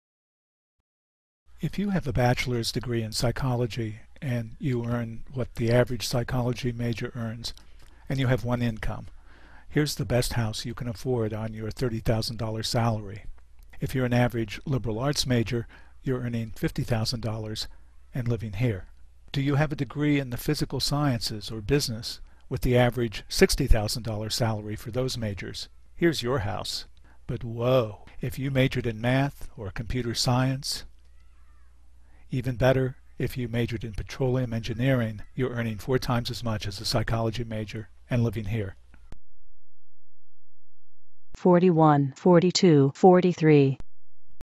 Mommas Don't Let Your Babies Grow Up to Be Psych Majors
If future earnings are important, you'd have to be crazy to become a psych major. A Georgetown study says some majors are good for four times as much money over a lifetime.